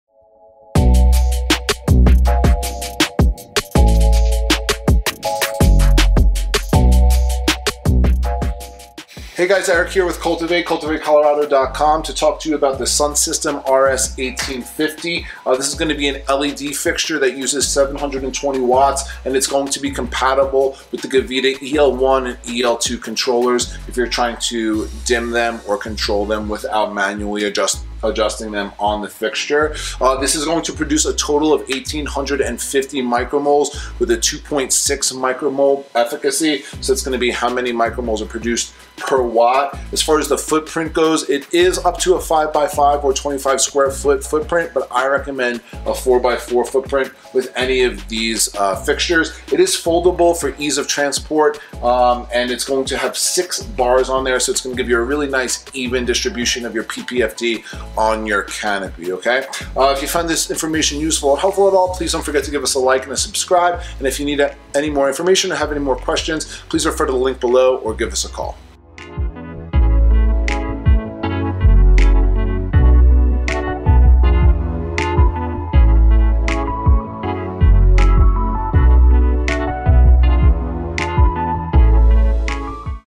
Sun System RS 1850 LED Grow Light Review and Info
The Sun System Rail System (RS) 1850 LED is a 6 rail, foldable LED producing 1850 μmol/s with an efficiency of 2.5 μmol/j. This fixture is DLC listed, IP65 wet-rated and comes complete with a manual digital dimming button and an internal embedded smart controller that is compatible with the Gavita e-Series Controller. Together, these features give you the ability to control up to 500 fixtures.